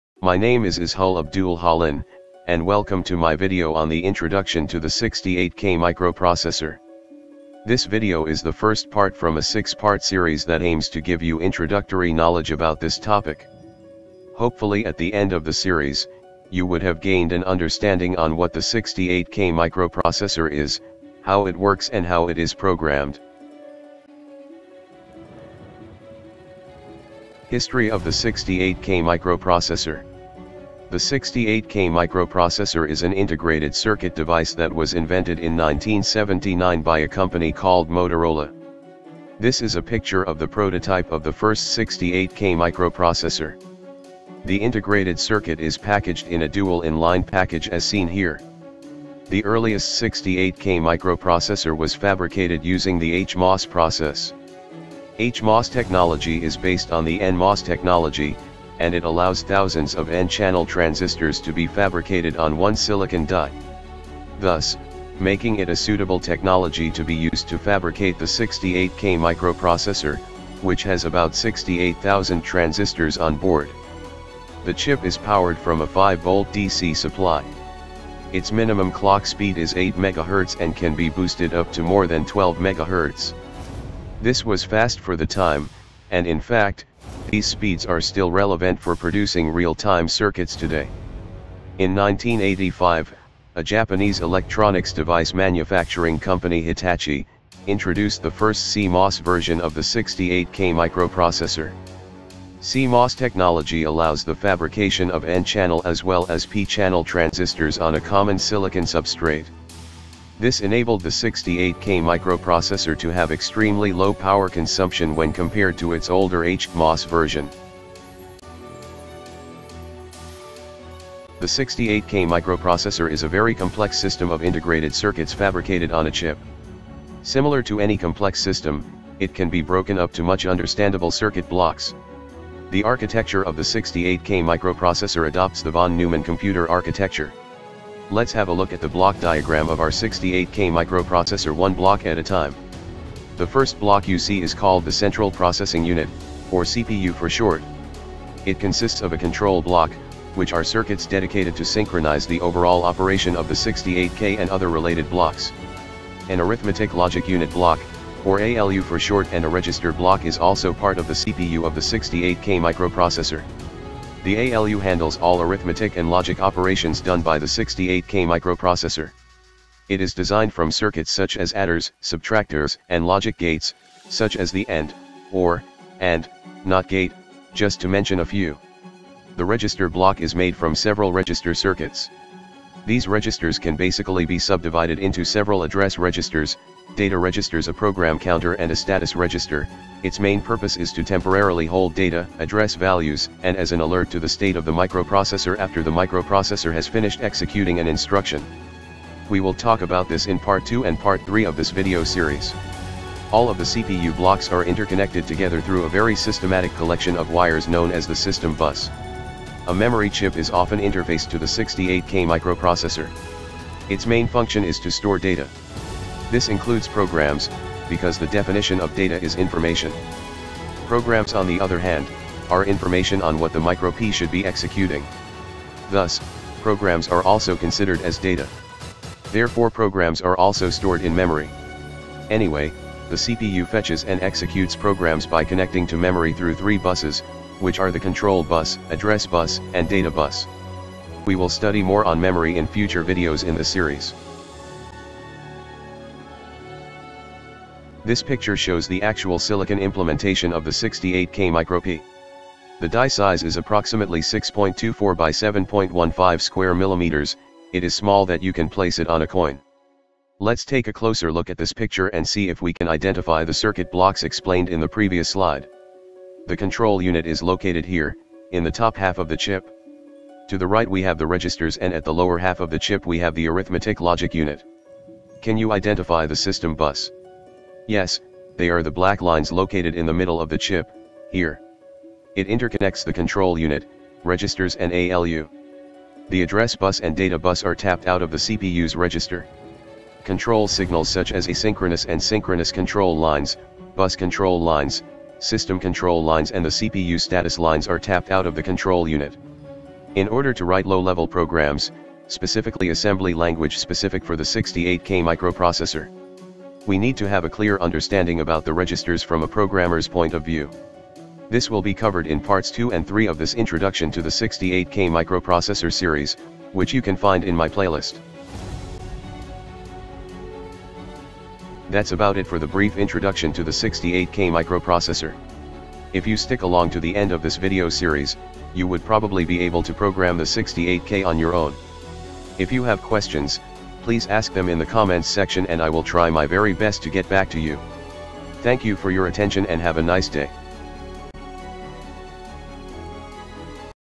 Intro to the 68k - PART 1 : Architecture
A little bit on the history and architecture of the 68k microprocessor is presented in this video. Enjoy.